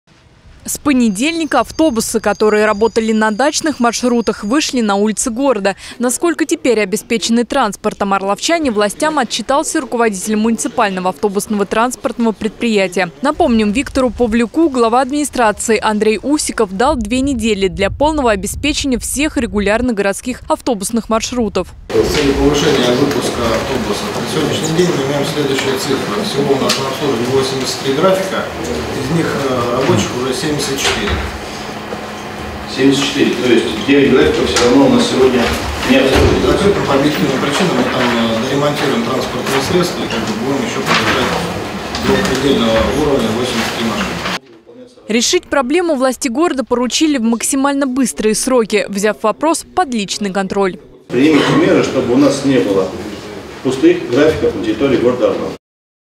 Дачные маршруты
С понедельника автобусы, которые работали на дачных маршрутах, вышли на улицы города. Насколько теперь обеспечены транспортом орловчане властям отчитался руководитель муниципального автобусного транспортного предпрития.